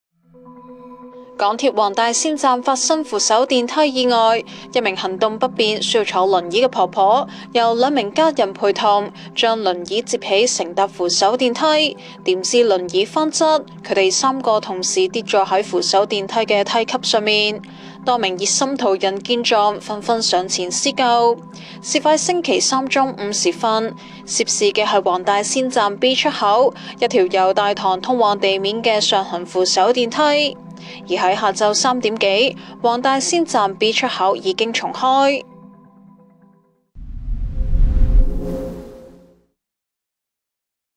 扶手梯意外｜輪椅翻倒婆婆一行3人受傷 熱手途人上前施援 星島頭條｜時事｜突發｜港鐵｜黃大仙站｜輪椅｜長者｜安全
港鐵黃大仙站發生扶手電梯意外。今日（24日）中午時分，一名行動不便需要坐輪椅的婆婆，由2名家人陪同將輪椅摺起，乘搭扶手電梯。未料期間輪椅翻倒，3人同時跌坐扶手電梯梯級上。多名熱心途人見狀紛紛上前施救。